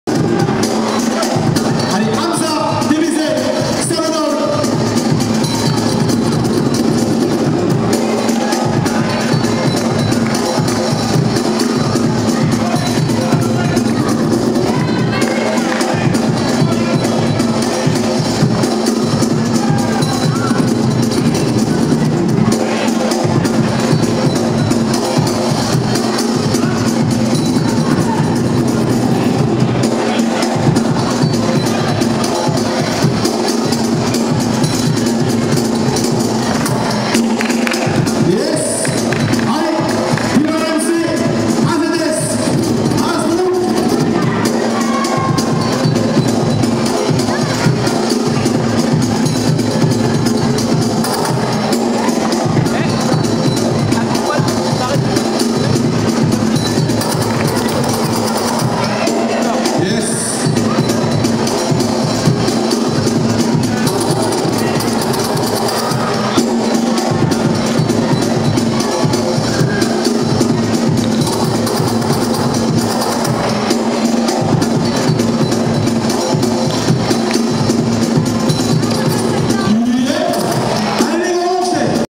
Battle Circus #2014 - Qualif. 1vs1 Ham'az (DBZ) vs LemC (AZS)
Phase de qualification

Orga : Merry Crew - AmenZulu Family
Deejay : Tbull
Speaker : Maleek